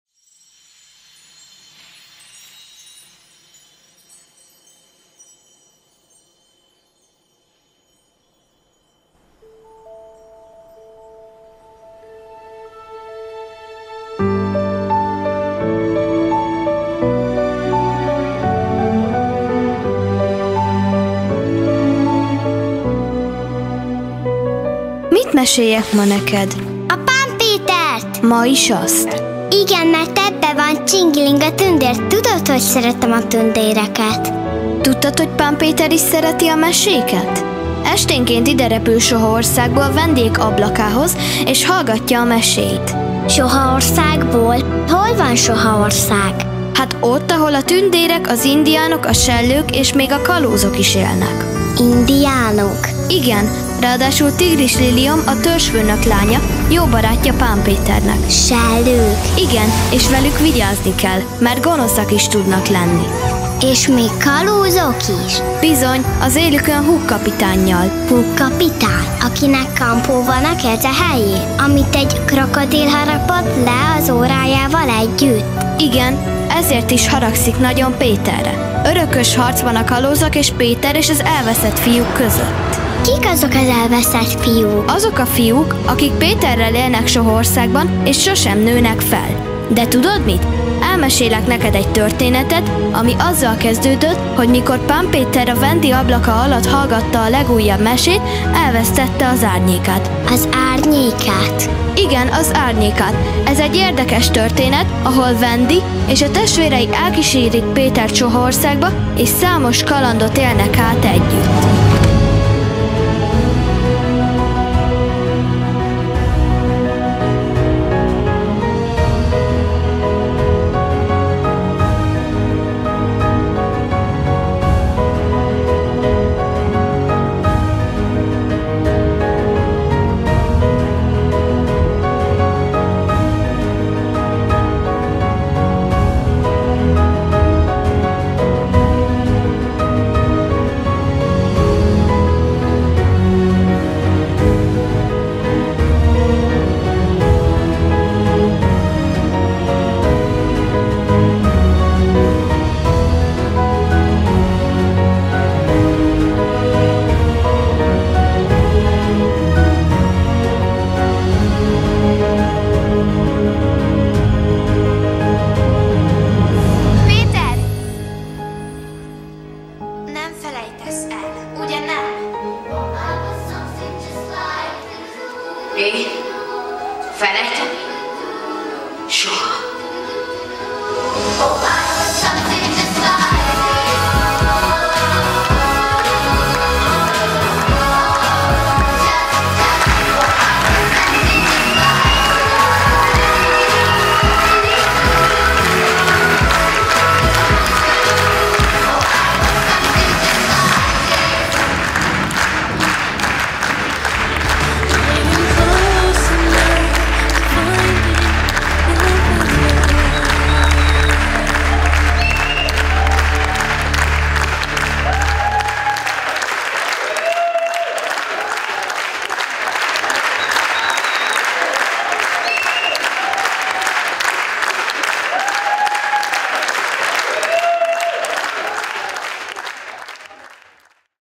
Pán Péter / Tánc és Légtánc mese előadás
Gyertek el velünk oda, ahol az álmok valóra válnak és soha nem érhetnek utol a felnőtt dolgok...

A NEW STEP Fitnesz és Táncstúdió legkiválóbb
táncosaiból áll táncszínházunk csapata.
A szereplők között tánc és légtánc
magyar-, európa- és világbajnokokat is
láthatunk.
A maga nemében egyedülálló produkció. A
szereplői gyerekek, akik profi módon
játszanak a színpadon.

A TÖRTÉNET ott kezdődik, hogy Pán Péter Csingilingel
Londonba repül, hogy aznap este is
meghallgassa Wendy meséjét. Azonban ez az
este kissé máshogy alakul, mivel Péter elveszíti az árnyékát. Ezzel elkezdődik Wendy és a testvérei kalandja Sohaországban.
Megismerkednek a Sellőkkel és Tündérekkel, barátságot kötnek az Indiánokkal és az Eltűnt Fiúkkal és megküzdenek a Gonosz Kalózokkal.

A SZÍNPADON a földön és a levegőben is látványos produkciókkal mesélik el a gyerekek a történetet. Több táncstílusból tevődnek össze a koreográfiák, melyeket akrobatika és légtánc elemek is színesítenek. Silken, hammockon, karikán, loopson és más egyedi eszközökön is megjelennek a szereplők.
Előadásunk gyerekeknek és felnőtteknek is hatalmas élményt nyújt.

SZEREPLŐK
Pán Péter: Dancz Boglárka
Wendy Darling: Zornánszki Hanna
Csingiling: Márton Hanga Lea
John Darling: Makai Sára
Michael Darling: Kepenyes Alexa
James Hook kapitány/Mr. Darling: Sánta Anna
Mr. Smee/Mrs. Darling: Deme Réka
Tigris Liliom: Barabás Zille
Árnyék: Szűcs Zille
Millicent Nagynéni: Majzik Enikő
Eltűnt fiúk:
Maródi Bori
Kristály Tamara
Kollár Csenge
Nagy-Hornok Olívia
Tóth Tamara
Tóth Lilien

Kalózok/Indiánok:
Szűcs Zille
Tóth Emese
Majzik Enikő
Zimányi Lili
Bancsó Eleonóra
Kepenyes Léna
Juhász Júlia
Erős Lili
Hegedűs Tímea
Mondi Lili
Tóth Blanka
Tolvaj Dorina
Nagy Dóra
Terék Dorina
Földi Melinda
Jánosi Virág
Fekete Blanka
Papp Regina
Kalcsó Nóra
Jánosi Virág
Zsúnyi Viktória
Zsúnyi Dorottya
Földes Hanna
Nemes Lea Zsolna
Kovács Olívia
Forgó Jázmin
Kézér Anna
Sellők:
Kepenyes Léna
Kollár Csenge
Kristály Tamara
Maródi Bori
Márton Hanga Lea
Szűcs Zille

Tündérek:
Karika:
Szűcs Zille
Márton Hanga Lea
Papp Regina
Füleki Nóra Léda
Lázár Maja
Malatinszki Laura
Tánc:
Sebők Melinda
Pataki Zsófia
Pataki Zsuzsanna
Lakatos Stella Szófia
Kárcis Kincső
Szűcs Árven
Barabás Lelle
Dézsi Écska
Petró Liliána
Szolnoki Réka
Szolnoki Sára
Szokolyi Zita
Silk:
Kepenyes Léna
Bancsó Eleonóra
Kézér Anna